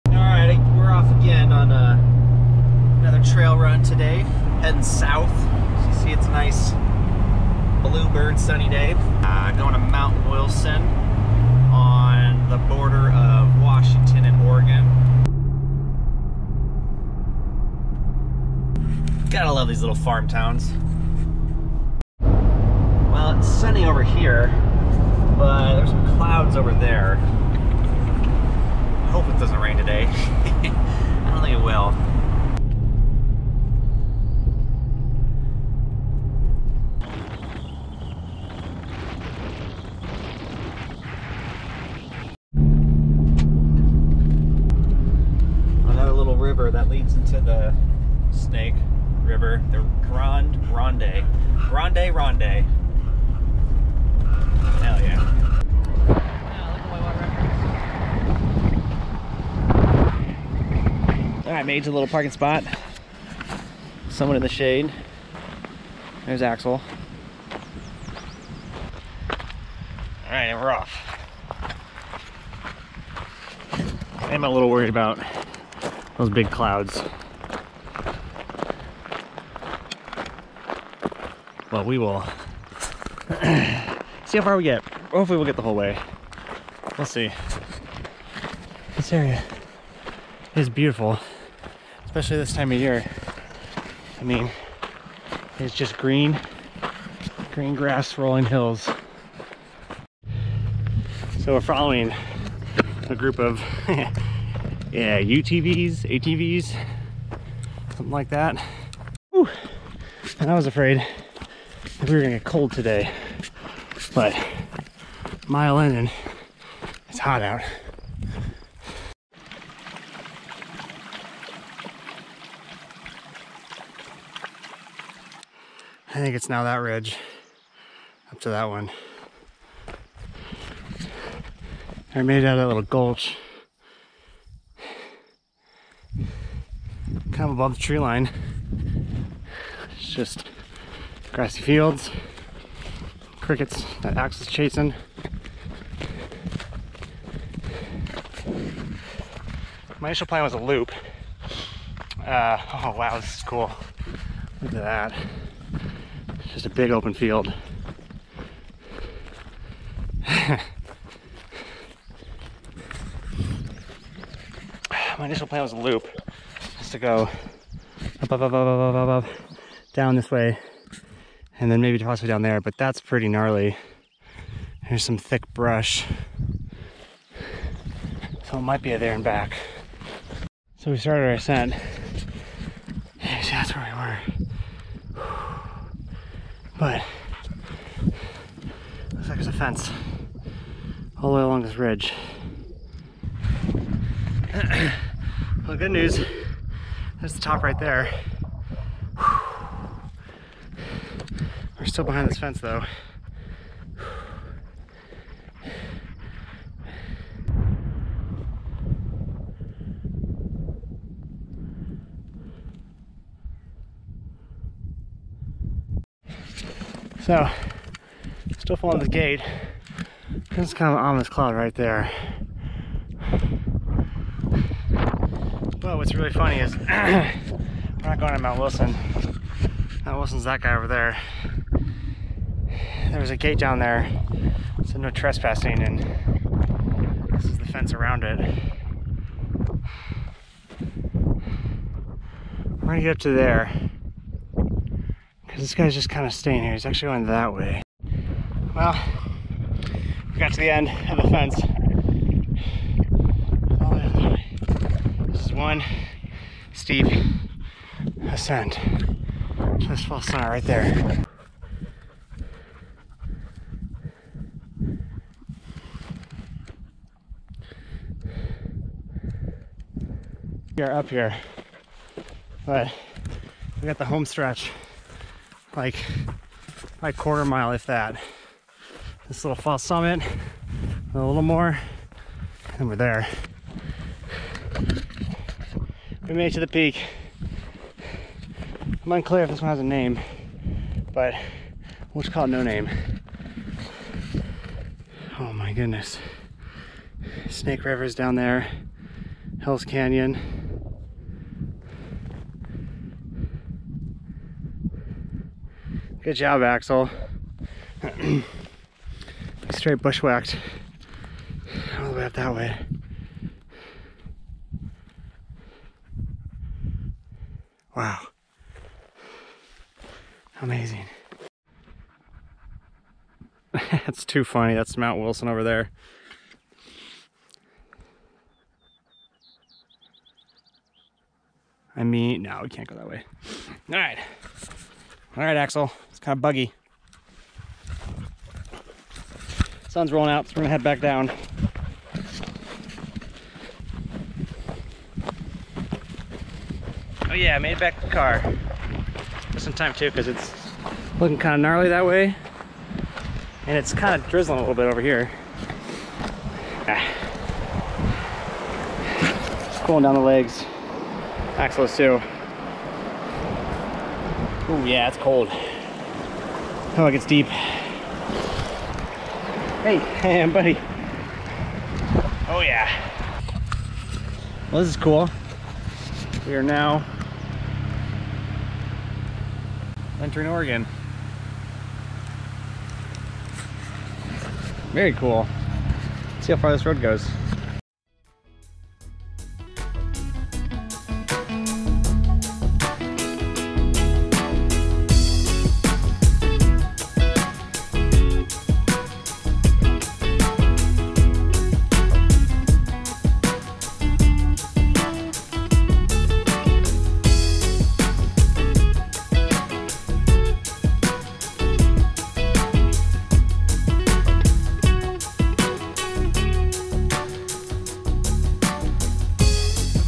Green Gulch Trail Run
Went for Mt Wilson and got a no named peak instead. Fun day